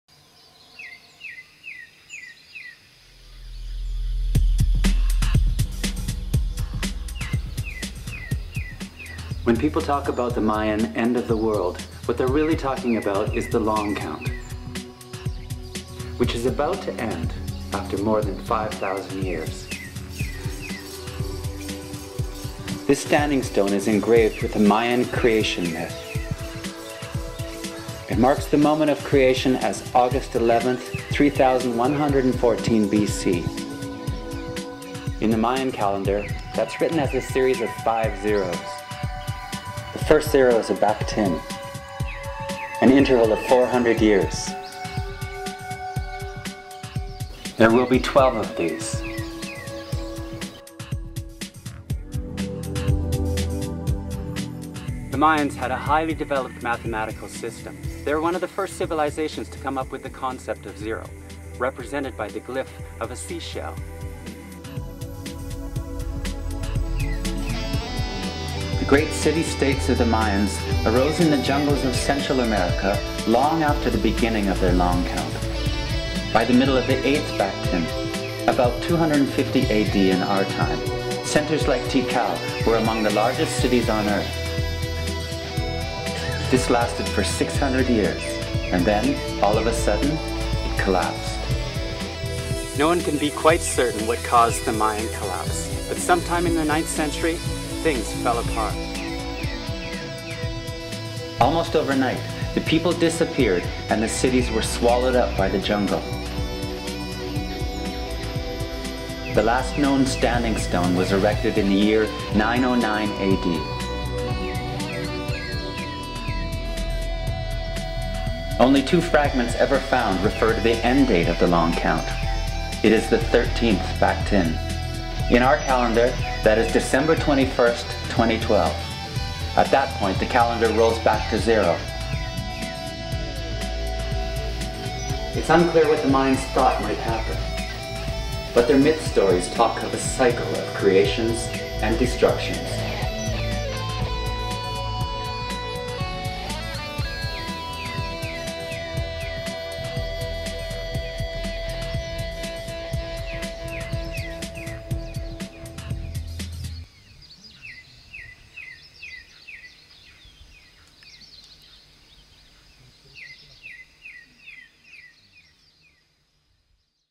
Mayan Calendar Explained
The Mayan calendar explained in less than three minutes.